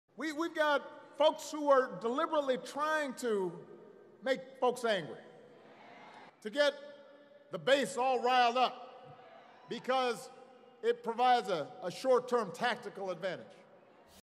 [VOCAB of the Day] get (all) riled up/ deliberately
* rile_ to make (someone) annoyed or irritated
* deliberately_ done in a way that was planned, not by chance

Source: CNN